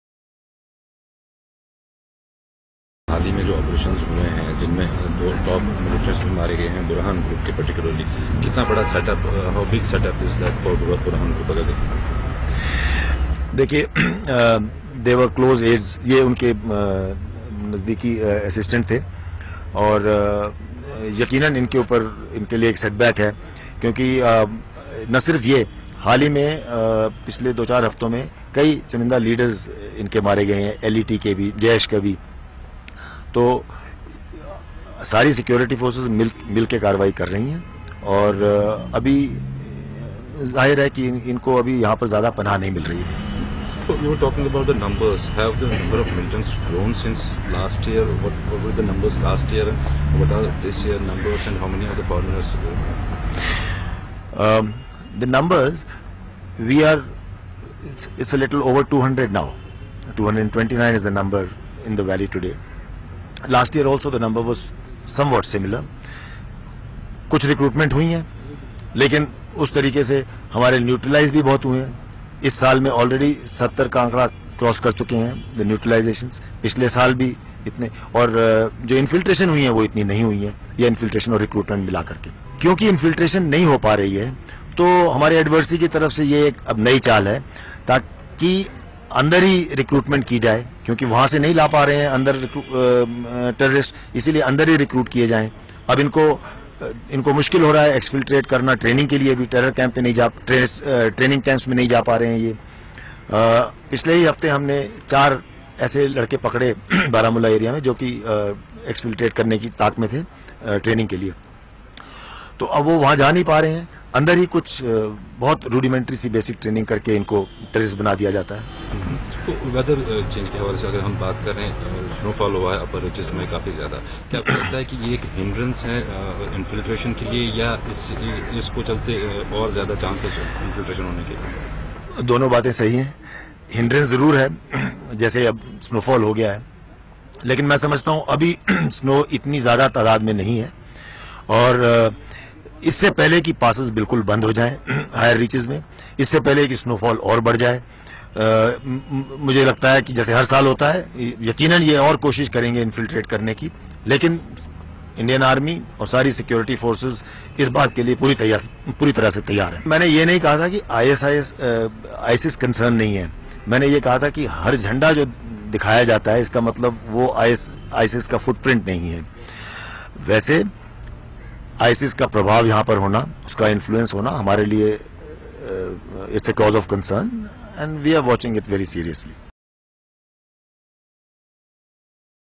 Over 200 militants waiting to infiltrate into India, says Army
Srinagar, Oct 25 (ANI): General Officer Commanding of the 15 Corps Lt General Satish Dua on Sunday said more than 200 militants had been waiting to infiltrate into India. Dua said in recent operations carried out by the Indian Army, many close aides of various terrorist organisations were killed. He added that the security forces had been working on it. He said as not much recruitment was done from across the border, terrorist organisations were recruiting cares from inside and training them.